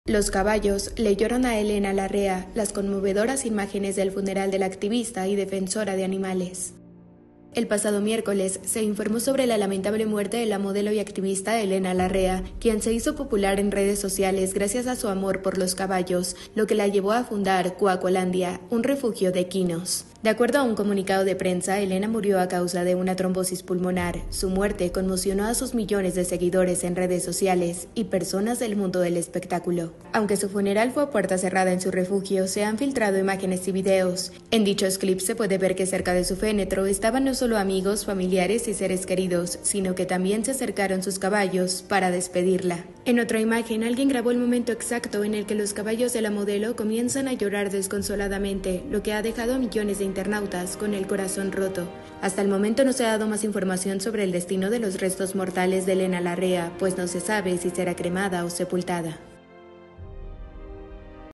Sus caballos se despidieron entre lágrimas, filtran fotos del funeral de Elena Larrea
Fue este miércoles 20 de marzo cuando se confirmó la inesperada muerte de la modelo y activista Elena Larrea quien se hizo popular en redes sociales gracias a su amor por los caballos, lo que la llevó a fundar “Cucolandia”, un refugio de equinos.

De acuerdo con el comunicado de prensa que se compartió, Elena murió a causa de una trombosis pulmonar el pasado martes 19 de marzo a las 15:30 horas; su muerte ha conmocionado a sus millones de seguidores y aunque su funeral se llevó a puerta cerrada en “Cuacolandia”, en redes ya circulan algunas imágenes del último adiós a la activista.